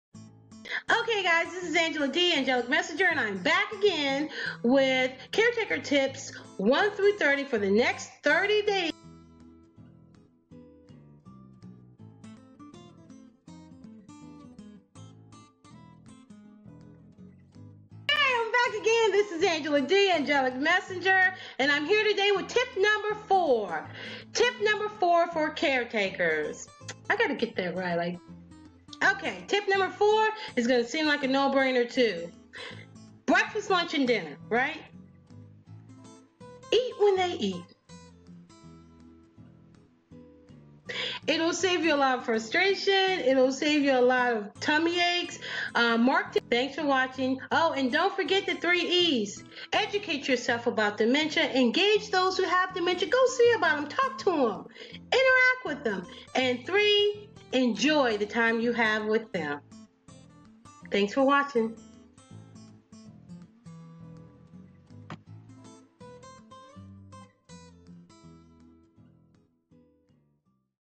LOVE, HONOR & DEMENTIA: 30 TIPS IN 30 DAYS FOR CAREGIVERS - Day 4
Hi, everyone.  My name is Angela "Angelic Messanger" Denmark.  I used to do natural hair videos, but life happened!  My mom was diagnosed with dementia in the Spring of 2013.  Natural hair took a back seat to mama's condition.  My nephew, Mark, and I  threw all we had into supporting her.  It's now five years down the line, and as her condition worsens and she enters the last stage, I thought I would put what we've learned into a quick, easy format to benefit others.  Use the tips.  They work!

THE THREE E'S OF DEMENTIA:
EDUCATE. ENGAGE. ENJOY.

SUBJECT LINE: Love, Honor & Dementia